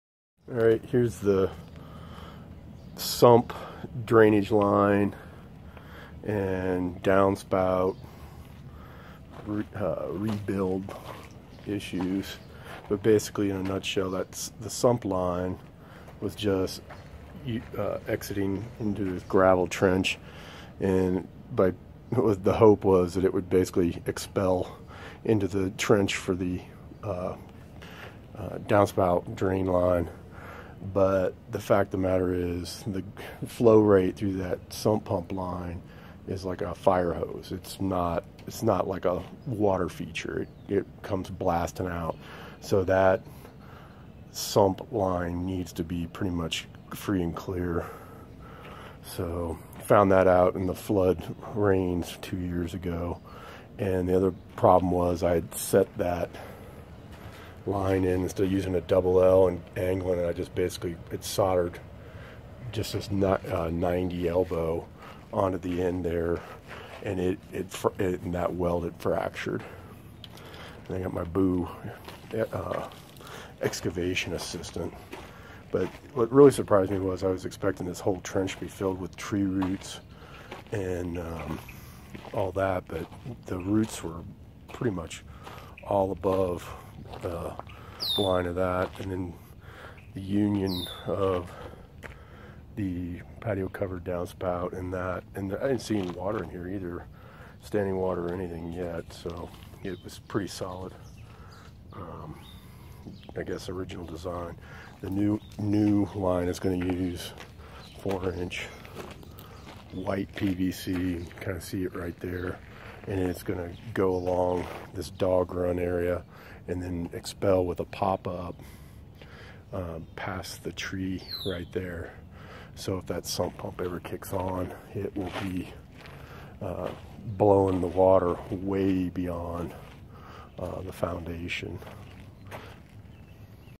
Down spout and sump pump drain line excavation
Excavation of existing down spout and sump pump drain line